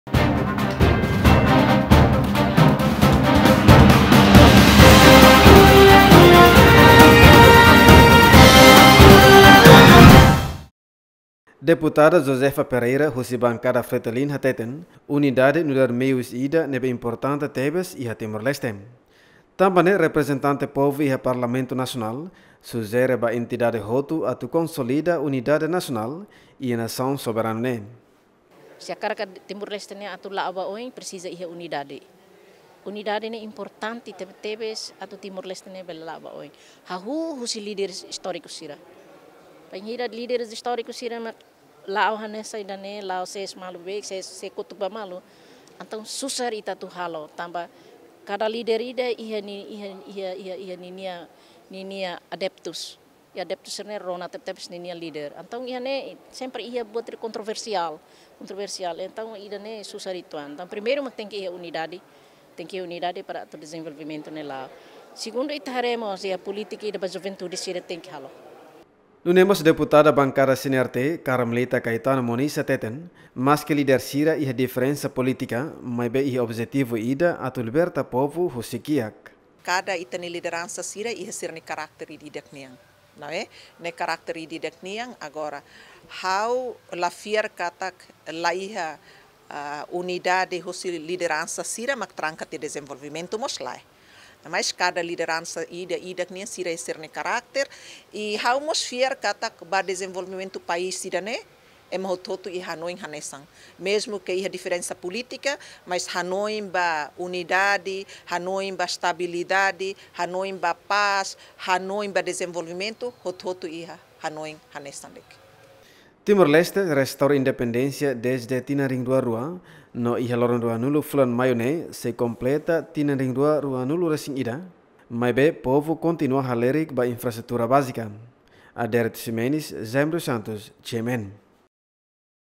Bankada FRETILIN: Atu aselera prosesu dezenvolvimentu rai laran presiza iha Unidade Nasional
Bankada FRETILIN konsidera atu aselera prosesu dezenvolvimentu rai laran presiza iha Unidade Nasional